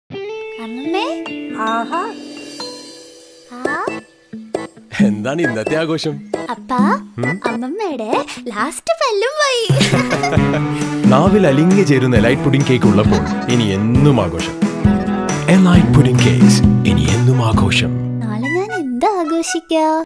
ELITE PUDDING CAKE - Malayalam
Treat your tastebuds to soft and indulgent pudding cake of dreams. Elite pudding cakes are made with the best of ingredients carefully chosen to bring to you superior quality and taste. Pick from an array of flavours and get ready to lose yourself.